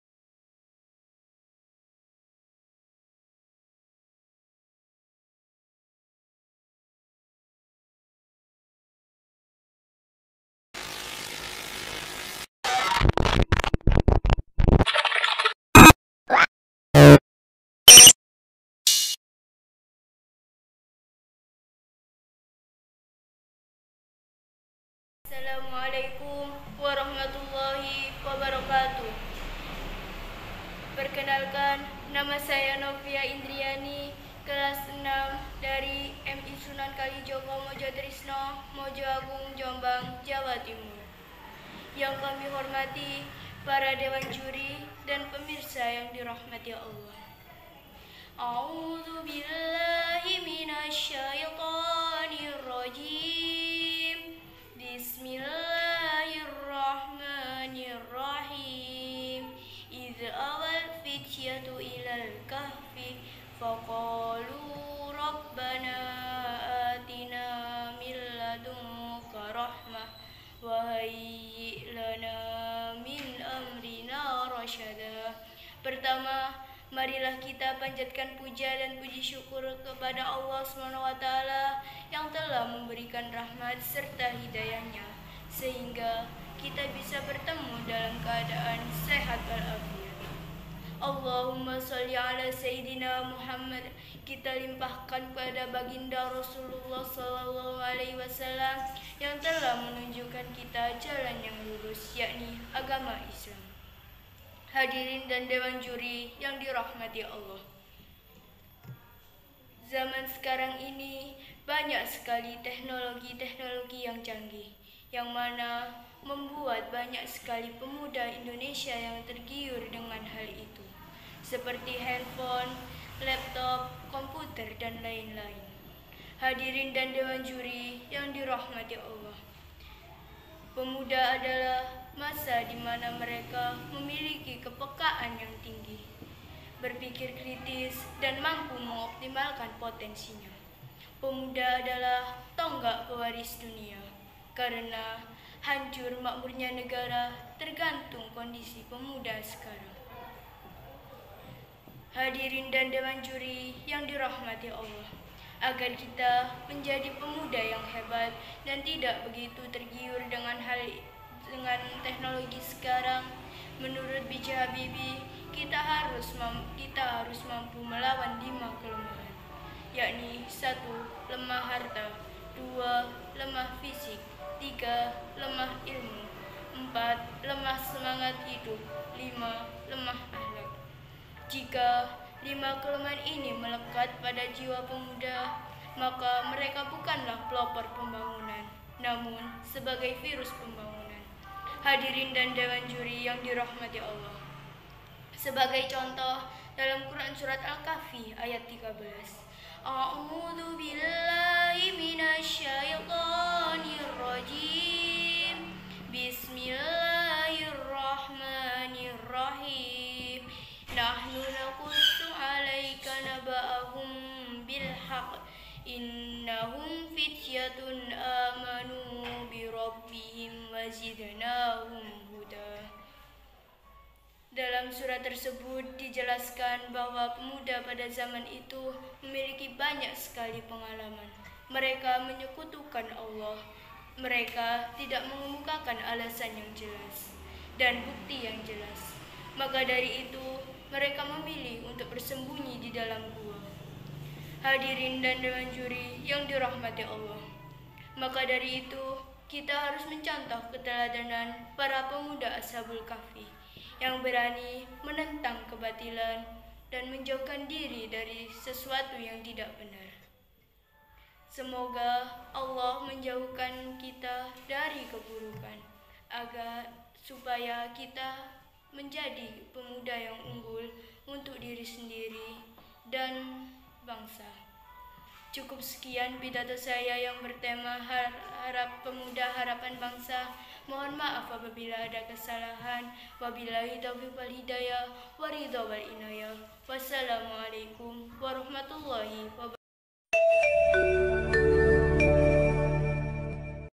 MDM05 / PIDATO B. INDONESIA / SD : NOVA INDRIANI, MI SUNAN KALIJOGO
MUSDALIN (Musabaqoh Darul Mubalighin) adalah sebuah ajang perlombaan islami tahunan yang diadakan oleh Pondok Pesantren Jabal An-Nur Al-Islami, yang melibatkan seluruh peserta tingkat SD/MI, SMP/MTs, SMA/MA Dan Umum.
ALAMAT :
Jl. W Rahman Kp. Parendoan Kel. Batu Putuk Kec. Teluk betung barat, Bandar Lampung, ,lampung 35239